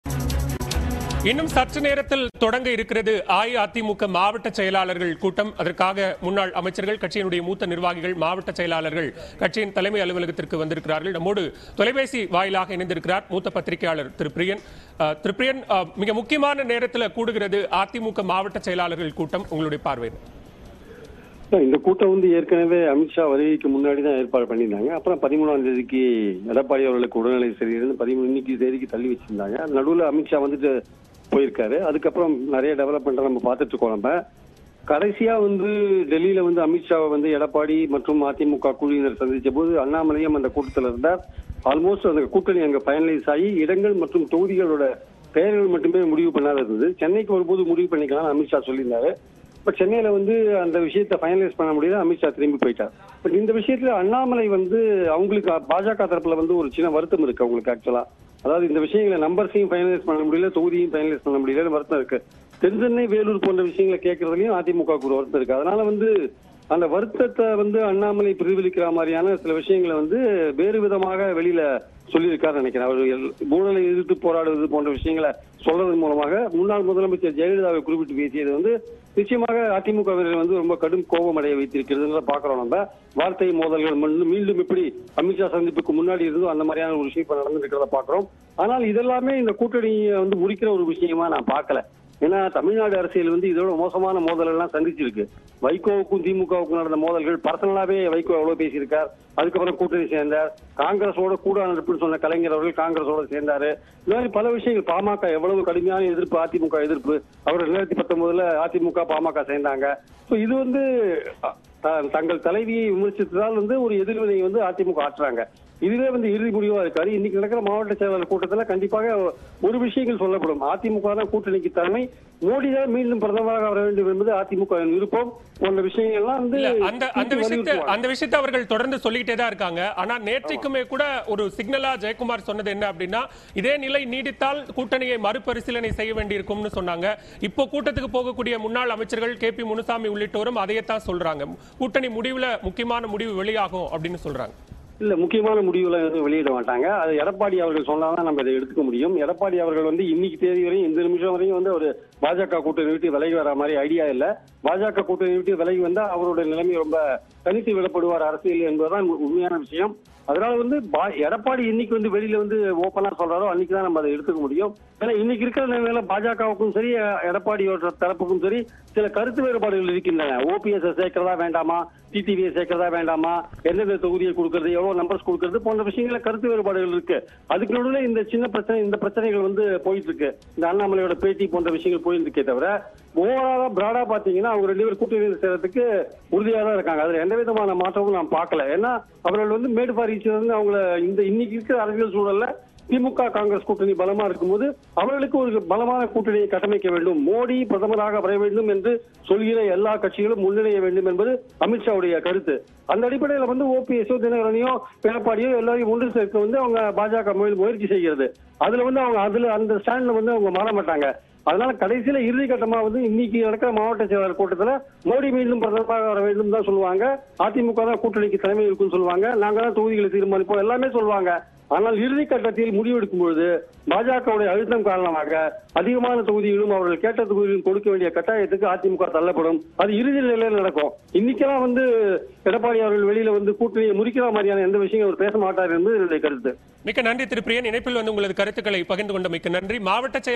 மோடி பிரதமராக வர வேண்டும் என்பது தான் அதிமுகவின் விருப்பம் - பிரியன், பத்திரிகையாளர் | AIADMK | BJP
மோடி பிரதமராக வர வேண்டும் என்பது தான் அதிமுகவின் விருப்பம் - பிரியன், பத்திரிகையாளர் | செய்திகளை துல்லியமாகவும் துரிதமாகவும் வழங்கும் தமிழகத்தின் முதன்மை செய்தி தொலைக்காட்சி உங்கள் நியூஸ்18 தமிழ்நாடு.

News18TamilNadu | AIADMK | BJP | Annamalai | Tamil News